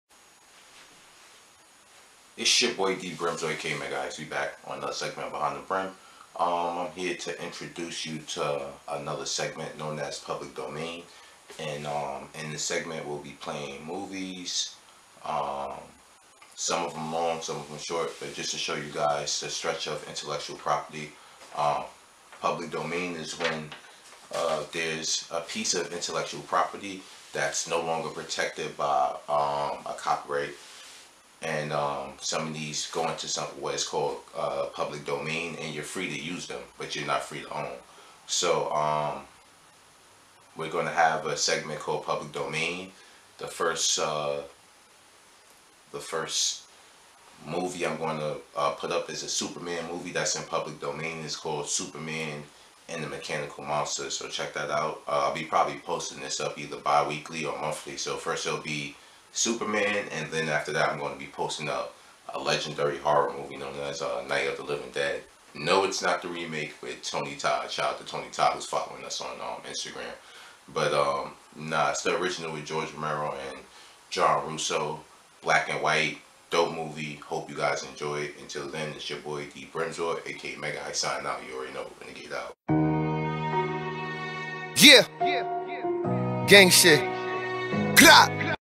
Introduction to Public Domain
On this segment of Behind The Brim, Dee Brimsorr introduces the audience to public domain and all it's benefits.